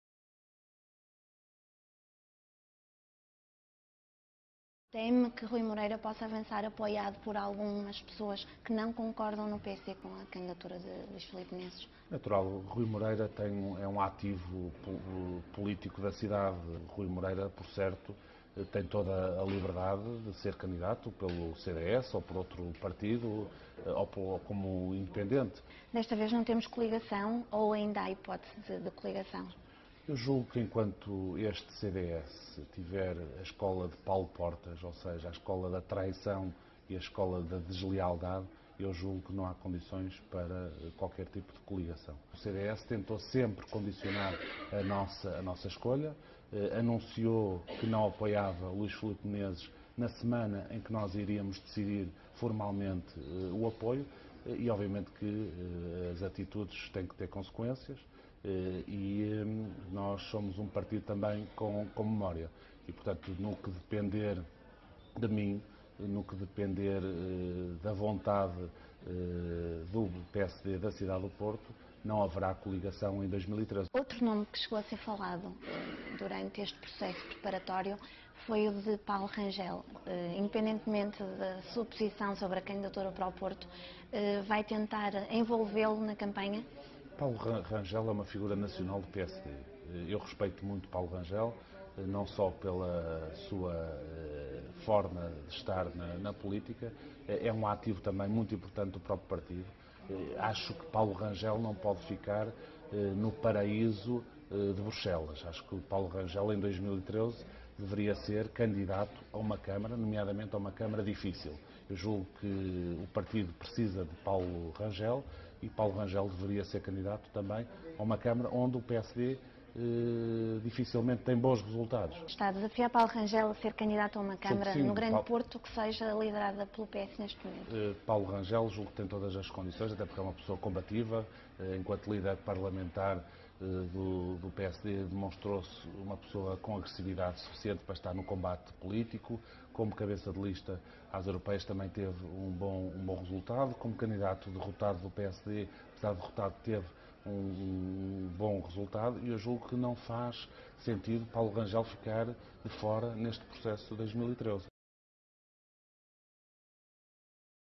"Escola de Portas é a da traição e da deslealdade"
O presidente da concelhia do PSD/Porto não poupa críticas ao CDS de Paulo Portas, culpando os sociais-democratas pelo fim da coligação para as autárquicas de 2013, em que o PSD vai apoiar a candidatura de Luís Filipe Menezes.